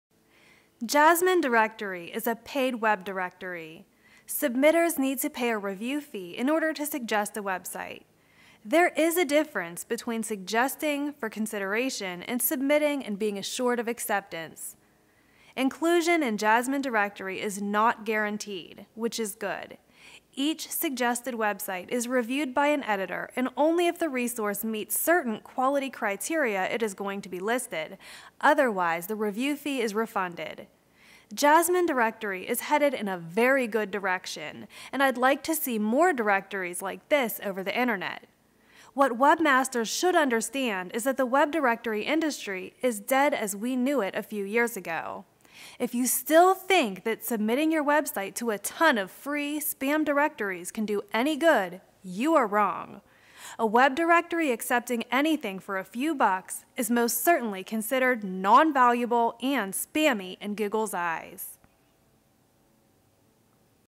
Jasmine Web Directory: Not just another website directory
This is a short review of Jasmine Business Directory located

In this short video users can learn the difference between what Google considers being a worthy web directory.

What takes apart this web directory from the crowd?

- Well, each main category and subcategory has its own unique article/description, making pages look more natural than just a bunch of hyperlinks.
- Relevant images in each category are also a nice touch.
- The business address appears in a Google generated map (this is useful especially for businesses), which, again, sets Jasmine Web Directory apart from other directories.
- As soon as a website is listed, the details can be easily edited by its owner.
- An upgrade feature is also available for those who want to modify their listing for whatever reason.
- They reserve the right to edit a listing, or even move it to another category (if needed). This isn't something I see too often, because, let's face it, a web directory owner will list a website although the title uses the same keyword several times instead of refunding the review fee.
- They apply a high editorial discretion and I personally haven't seen spammy titles and descriptions, which is again a good consideration for a directory as Matt Cutts clearly stated Google's view about paid web directories.
- A rejection list is also made public.